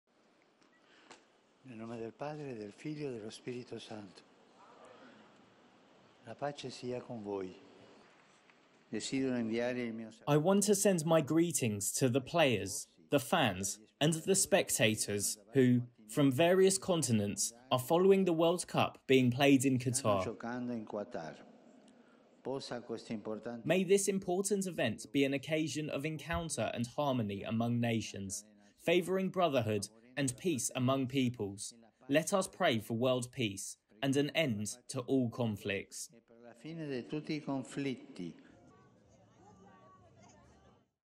Papa Francisc speră ca Cupa Mondială să aducă pacea
Papa Francisc și-a exprimat speranța că Cupa Mondială de fotbal din 2022 se va dovedi a fi un "prilej de întâlnire și armonie între națiuni, favorizând fraternitatea și pacea între popoare".

Vorbind la finalul audienței generale săptămânale din Piața Sfântul Petru, Papa le-a cerut, de asemenea, credincioșilor să se roage pentru pace și pentru încetarea tuturor conflictelor.

Francisc a fost ales în 2013 ca primul papă din America Latină și este un fan de-o viață al echipei de fotbal San Lorenzo din Buenos Aires.

Cupa Mondială FIFA 2022 se desfășoară în Qatar în perioada 20 noiembrie - 18 decembrie.